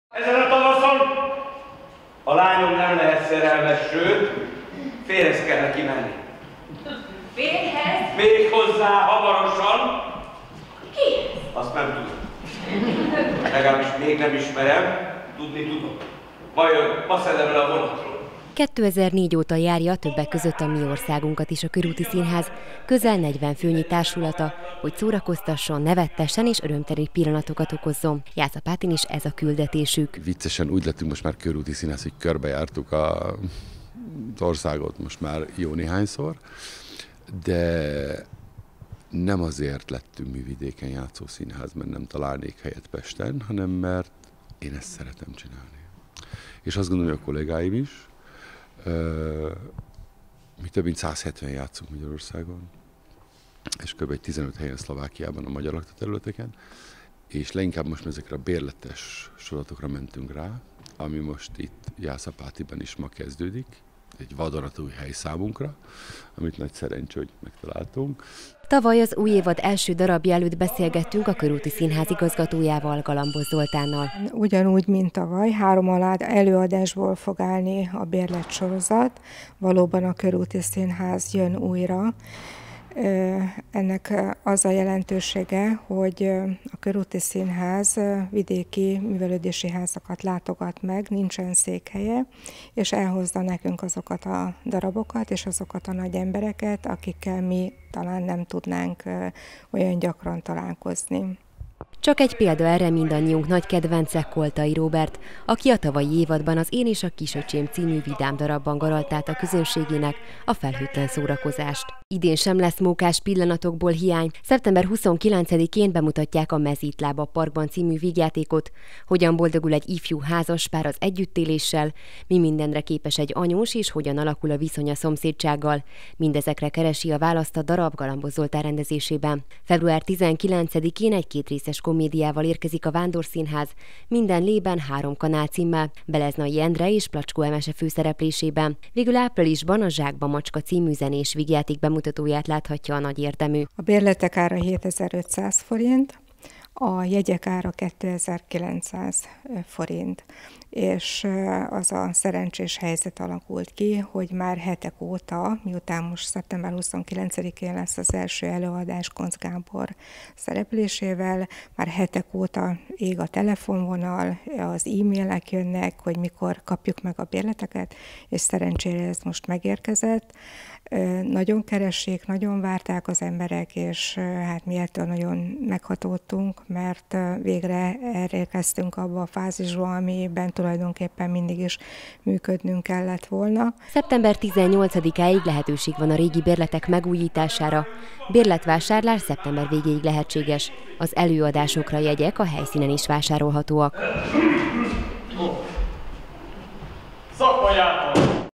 HAMAROSAN INDUL AZ ÚJ SZÍNHÁZI ÉVAD A JÁSZAPÁTI MŰVELŐDÉSI HÁZBAN
A jászapáti Pájer Antal Művelődési Ház közel két hónapja értékesíti a 2018/2019-es évadra összeállított színházbérletét. A remek előadások az új évadban is a Körúti Színház jóvoltából valósulnak meg. Az első előadásra már nem kell sokat várni, két hét múlva Koncz Gábor, Pikali Gerda és Gyebnár Csekka főszereplésében bemutatják a Mezítláb a parkban című vígjátékot.